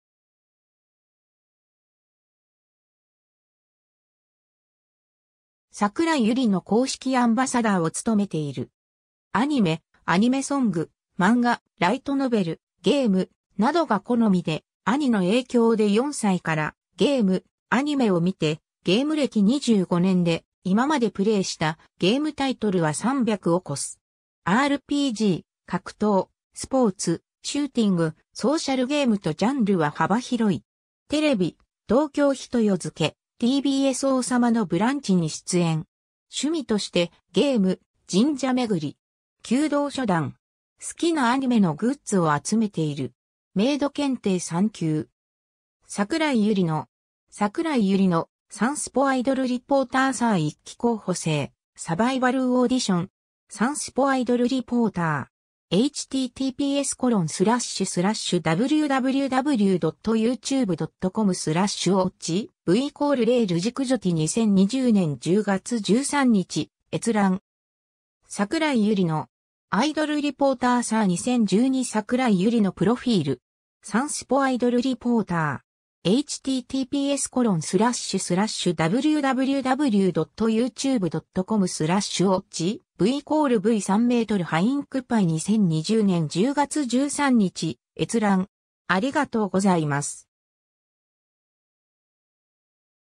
櫻井ゆりの
櫻井 ゆりの（さくらい ゆりの) は日本の女性タレント、ゲームタレント、ゲーム実況者。元王様のブランチリポーター、京都府出身。 2020年よりエヴァンゲリオン バトルフィールズ（EVABF）公式アンバサダーを務めている。
 アニメ、アニメソング、漫画、ライトノベル、ゲーム、などが好みで、兄の影響で4歳からゲーム、アニメを見て、ゲーム歴25年で今までプレイしたゲームタイトルは300を超す。
RPG、格闘、スポーツ、シューティング、ソーシャルゲームとジャンルは幅広い。
 テレビ東京一夜づけ、TBS王様のブランチに出演。
趣味としてゲーム、神社巡り。
弓道初段。
好きなアニメのグッズを集めている。
メイド検定3級。
^ 櫻井ゆりの (2011年10月16日). 櫻井ゆりの｜サンスポアイドルリポーターSIR1期候補生 ～サバイバルオーディション～. サンスポアイドルリポーター.. 2020年10月13日閲覧。
^ 櫻井ゆりの (2012年4月10日). アイドルリポーター SIR2012 櫻井ゆりの プロフィール. サンスポアイドルリポーター.. 2020年10月13日閲覧。